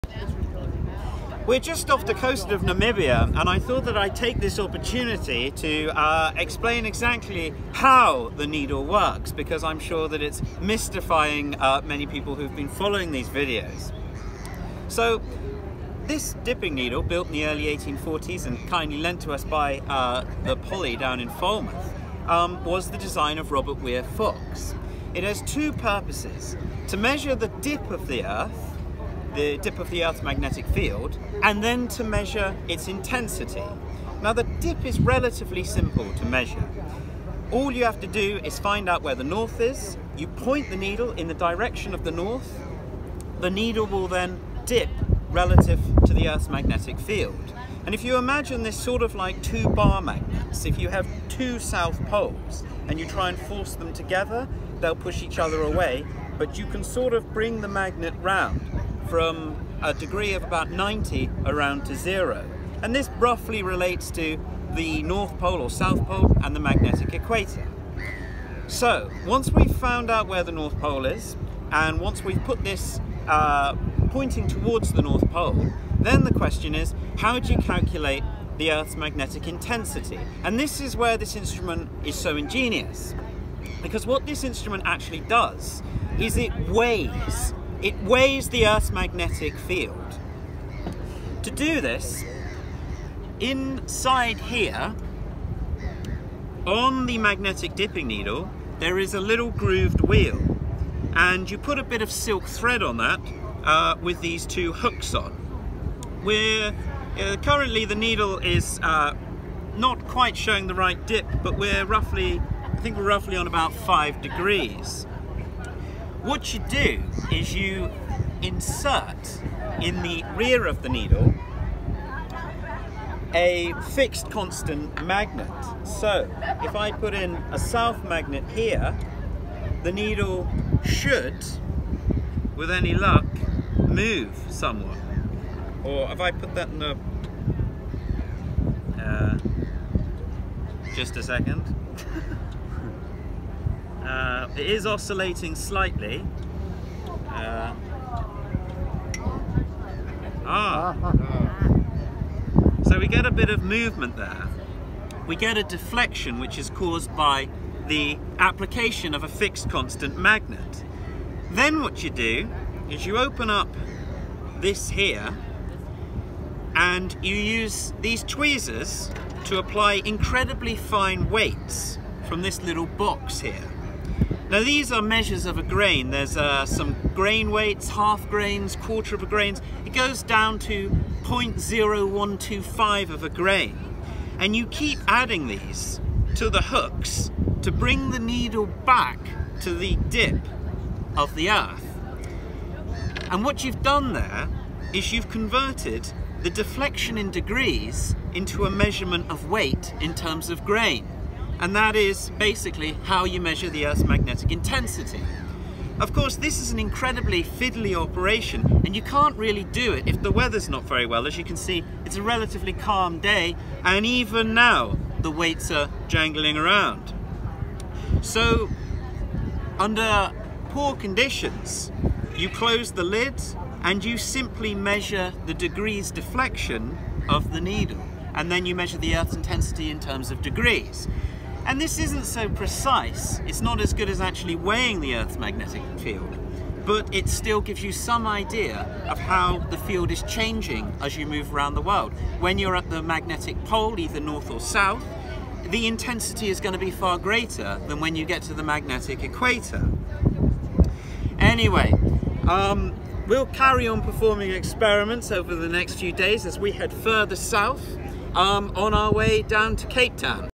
Weighing the earth’s magnetic field with Fox’s dip circle.
Weights and deflectors. Performing experiments with Robert Fox’s magnetic dip circle, off the coast of Namibia. Thanks to the Poly at Falmouth for looking after this beautiful instrument and all its kit for 130 years! Performed January 2020. Carried by HMS Terror and Erebus.